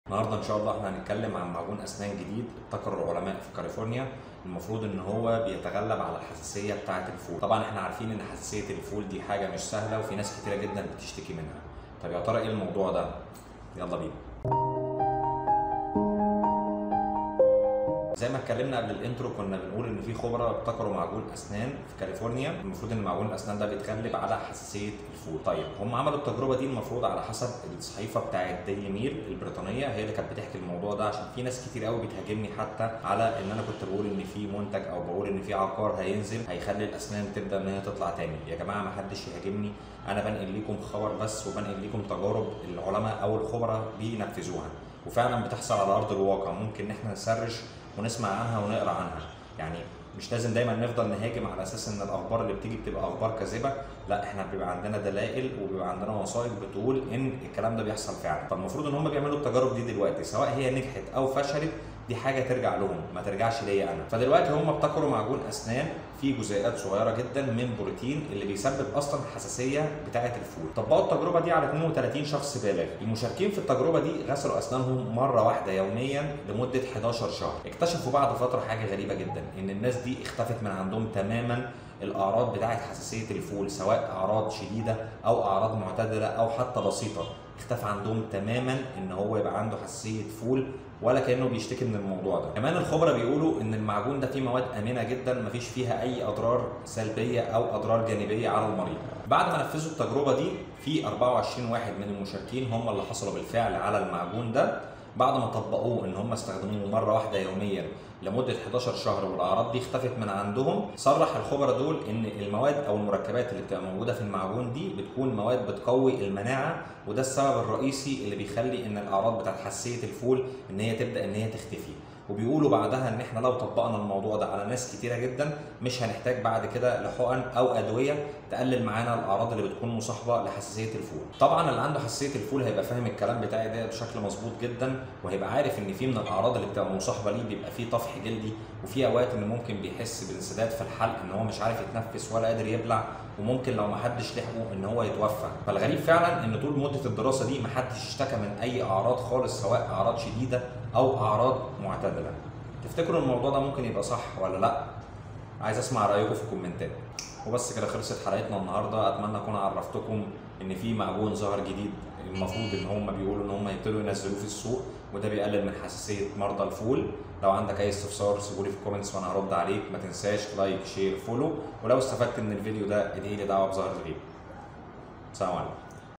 ابتكار معجون أسنان يخلصك من حساسية الفول
ابتكر العلماء في كاليفورنيا معجون أسنان جديد وآمن للاستخدام يساعد على علاج حساسية الفول السوداني، والتي تهدد حياة الملايين.

واختبر العلماء هذا المعجون الذي يحتوي على جزيئات صغيرة من بروتين الفول السوداني المسبب لهذه الحساسية عند الأشخاص، على 32 بالغًا يعانون بالفعل من حساسية الفول السوداني، حسبما نشرت صحيفة "ديلي ميل" البريطانية.

وقام المشاركون بغسل أسنانهم بهذا المعجون مرة كل يوم لمدة 11 شهرًا، ولاحظ الخبراء أنهم لم يعانوا بعد من حساسية الفول السوداني، مؤكدين أن تلك المواد المستخدمة بالمعجون آمنة.

وحصل 24 مشاركًا على هذا المعجون، بينما حاز 8 فقط على الدواء الوهمي، وكانت المجموعة الأولى قد ظهر عليها انخفاض أعراض حساسية الفول السوداني.

ويتكون معجون الأسنان الجديد من مركبات تدخل للفم وتقوي المناعة، وبالتالي يمكن للشخص غسل أسنانه مرة واحدة في اليوم، ولا يحتاج إلى حقن، كذلك لا يحتاج لتناول أي شيء لتقليل أعراض حساسية الفول السوداني.

وخلال مدة الدراسة التي وصلت 48 أسبوعًا، لم يبلغ أحد من المشاركين عن أي أعراض حادة أو معتدلة لحساسية الفول السوداني بعد غسل أسنانهم بهذا المعجون الذي يحتوي على جرعة عالية من بروتين الفول السوداني.

الجدير بالذكر أن حساسية الفول السوداني تعد من أكثر الأنواع شيوعًا، والتي تسبب طفحًا جلديًا يظهر على الجلد وانسداد في الحلق بشكل قد يجعل من الصعب التنفس، وربما تؤدي إلى الوفاة في حال عدم إنقاذ المصاب فورًا.